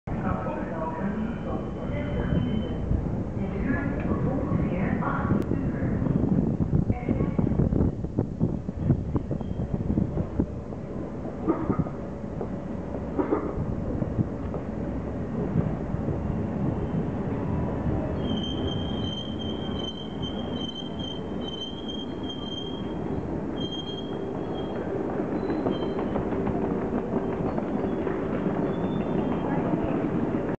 ICMm komt aan op station Amersfoort Centraal !
Op station Amersfoort Centraal arriveert een ICMm als IC van Rotterdam Centraal naar Assen!